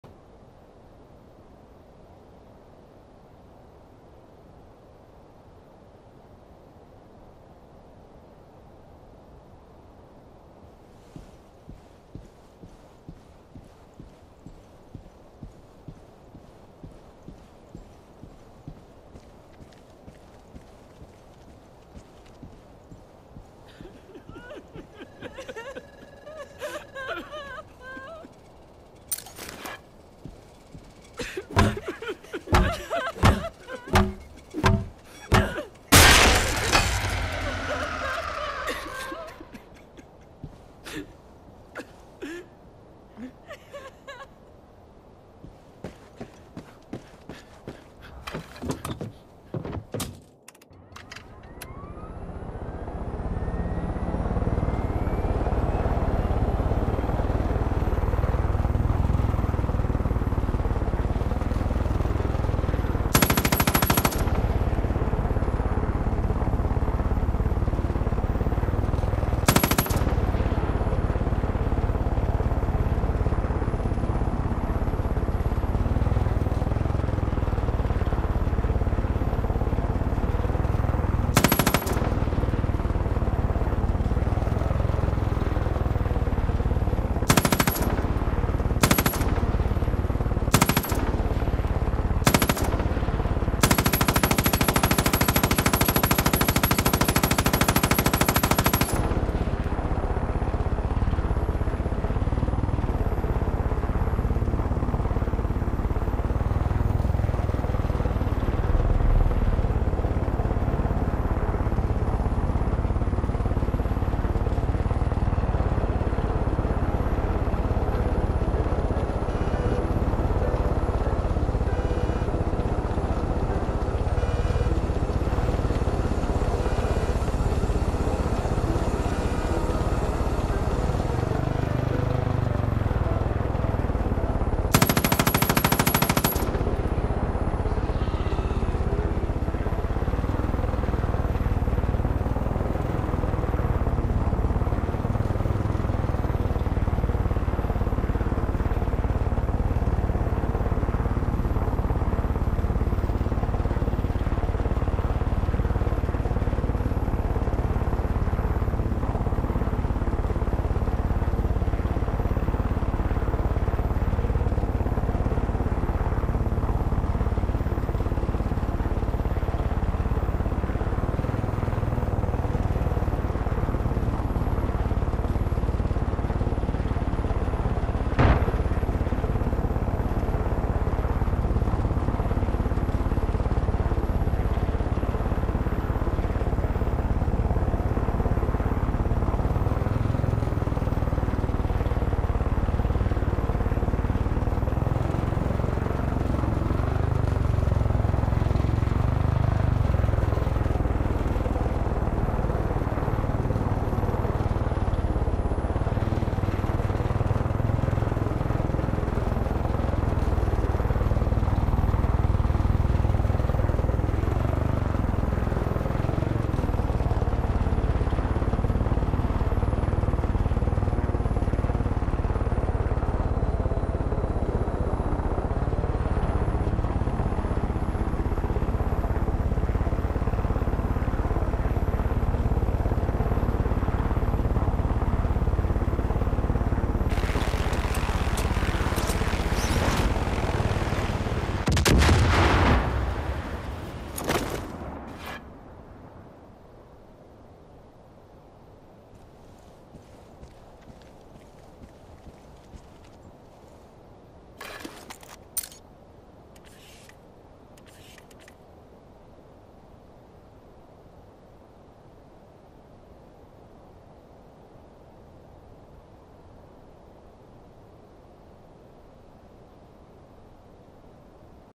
V Editor modtool
V Editor modtool for playstation 4/5 map editors. Mod Creator : Steve 64B .Editor creator playstation 4/5 : Restylex (for own use ) Will be updated by tutorial use.